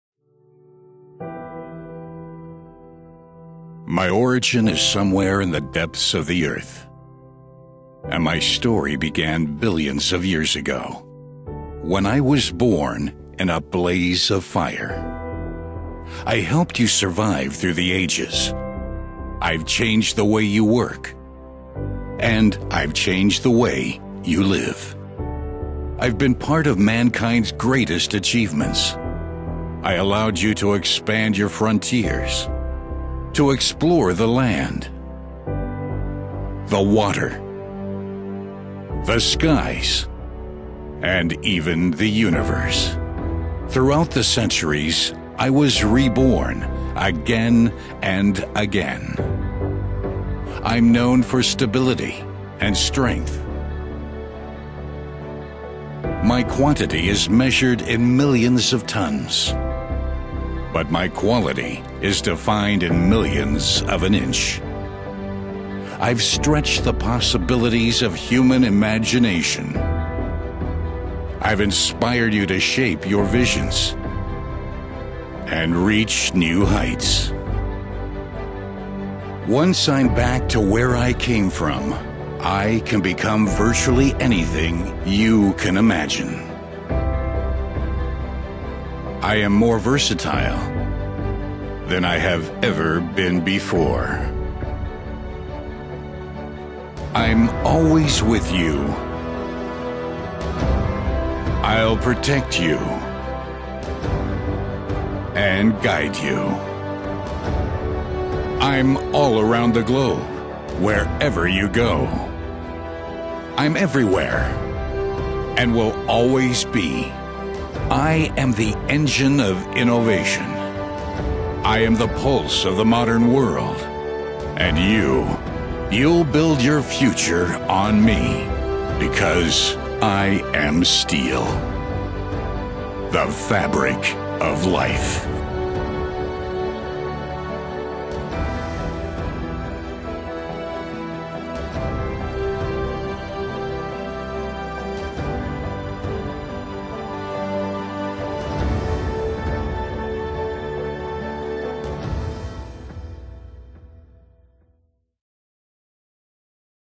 Acero, el tejido de la vida
Descubre los orígenes del acero que producimos, su historia a través de los siglos y la versatilidad que hace que los productos siderúrgicos tengan un papel clave en nuestro día a día.

El mundo moderno se apoya en el acero para la construcción de sus infraestructuras de carreteras, ferroviarias y energéticas, sus edificios, sus equipamientos domésticos, sus envases y sus vehículos. El acero es un material esencial en el mundo que nos rodea y desempeña un papel fundamental para ayudarnos a forjar un futuro más sostenible.

Descubre más acerca de cómo el acero constituye realmente el tejido de la vida moderna